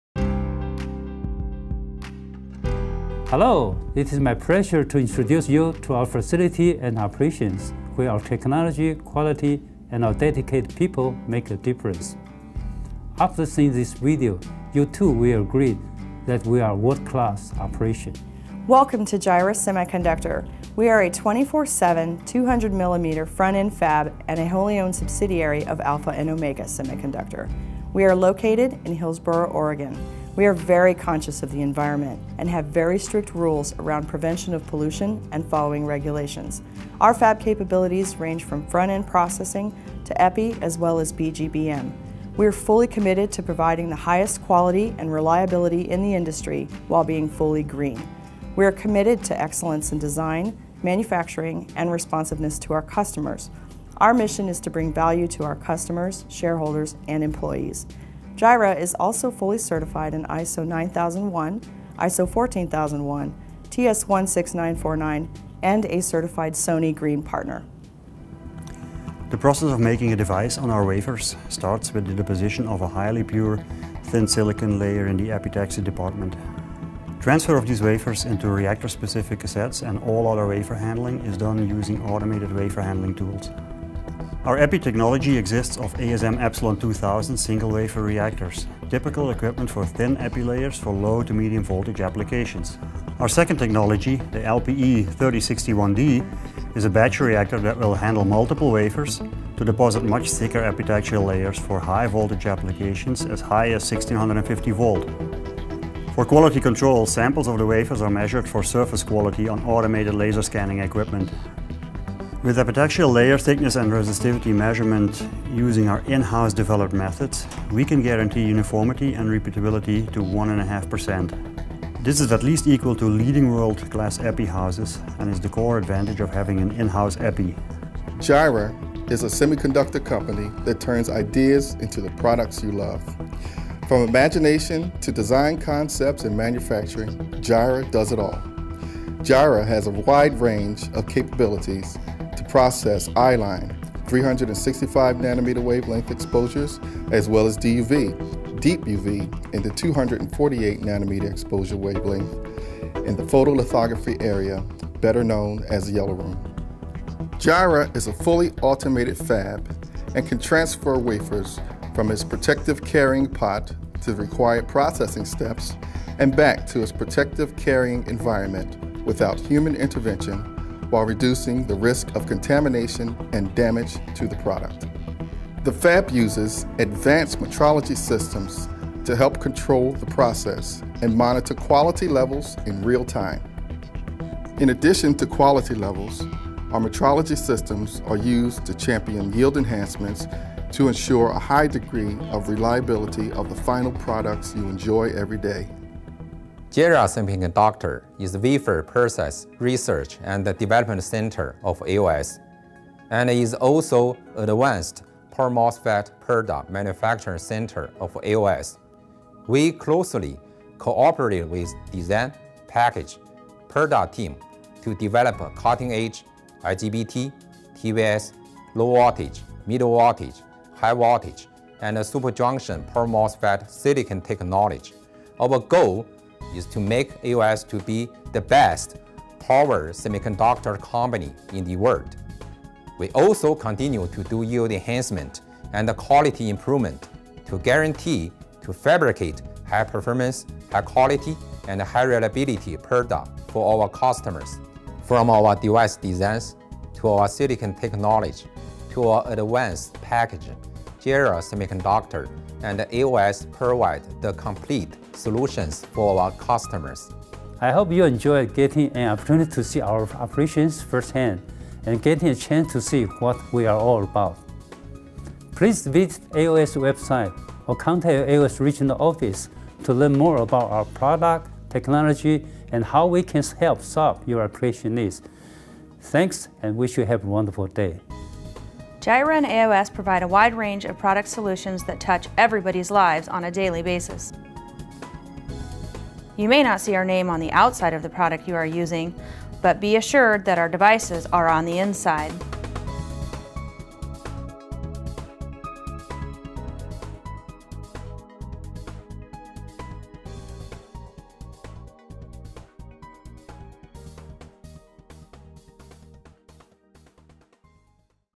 Jireh Semiconductor
Jireh Semiconductor, a wholly-owned subsidiary of Alpha and Omega Semiconductor Limited (“AOS”), produces semiconductors for computers, consumer electronics and communications devices.

Headquartered in Hillsboro, Oregon, Jireh Semiconductor is very conscious of the environment and has very strict rules around prevention of pollution and  following regulations.  Jireh's capabilities range from front end processing, EPI as well as BGBM.

Jireh is also fully certified in ISO 9001 / ISO 14001 / TS16949 and certified a Sony Green Partner.

Video produced by:  Michael Armstrong, Jireh Semiconductor
Video shot and edited by: Curtis Ellzey
for Jireh Semicondcutor